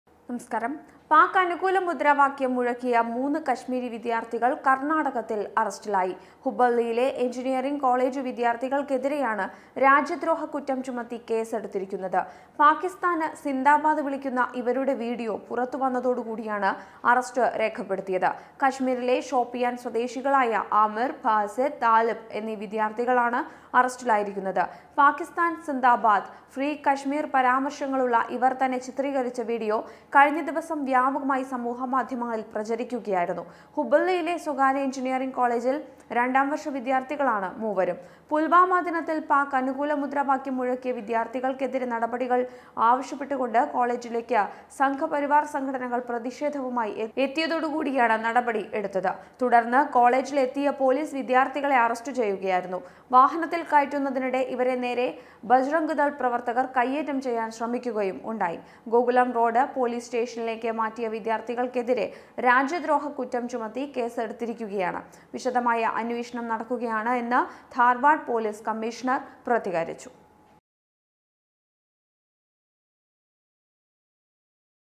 പാക് അനുകൂലം: 3 വിദ്യാര്‍ത്ഥികള്‍ അറസ്റ്റില്‍ I Jammu & Kashmir
പാക് അനുകൂല മുദ്രാവാക്യം മുഴക്കി; മൂന്ന് കശ്മീരി വിദ്യാർത്ഥികൾ കർണാടകത്തിൽ അറസ്റ്റിൽ; രാജ്യദ്രോഹക്കുറ്റം ചുമത്തി കേസെടുത്തത് ഹുബ്ബള്ളിയിലെ എഞ്ചിനീയറിങ് കോളേജ് വിദ്യാർത്ഥികൾക്കെതിരെ; പാക് അനുകൂല മുദ്രാവാക്യം വിളിക്കുന്ന വീഡിയോ വൈറൽ